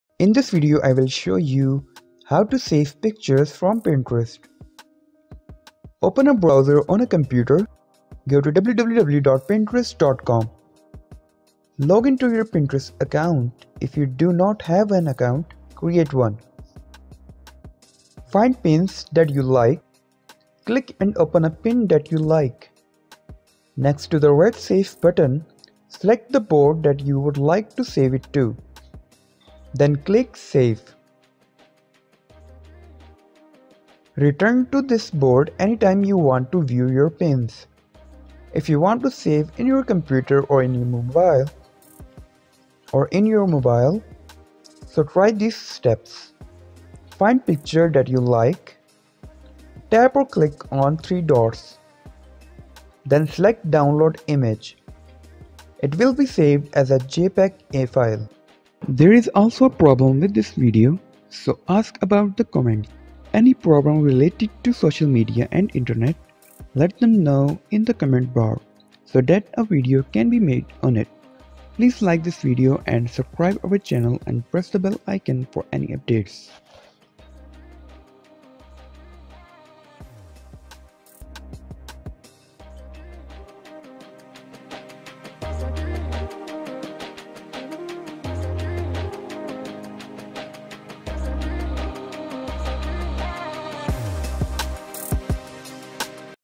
How to Save Pictures from Pinterest on Laptop | Do It Yourself.
In This Video I Will Show You How to Save Pictures from Pinterest.

How to Save Images From Pinterest.
Summery:
Open a browser on a computer.
Login to your Pinterest account. If you do not have an account, create one.
Find pins that you like.
Click & Open a pin that you like.
Next to the red Save button, select the board that you would like to save it to. Then, click Save.
Return to this board anytime you want to view your pins.
If you want to save in your computer or in mobile. So try these steps.
Find Picture that you like.
Tap or click on …, then select Download Image.
It will be saved as a JPEG file.